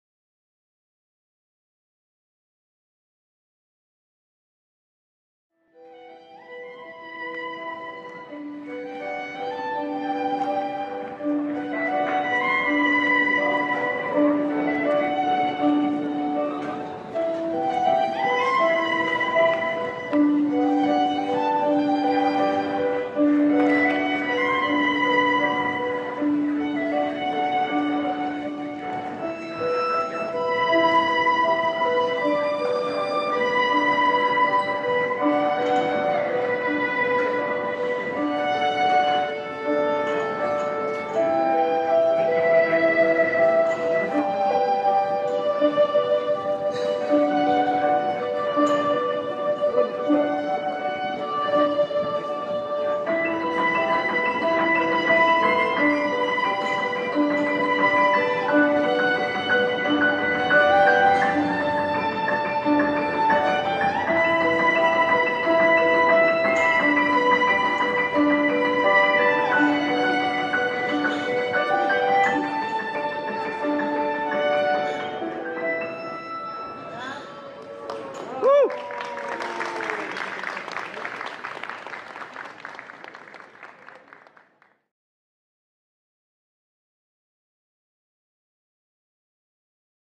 Quando Chiara.. Incontra la Musica!!
Era lì che giocava con i suoi amici quando.. Ad un tratto è partita la musica e Lei...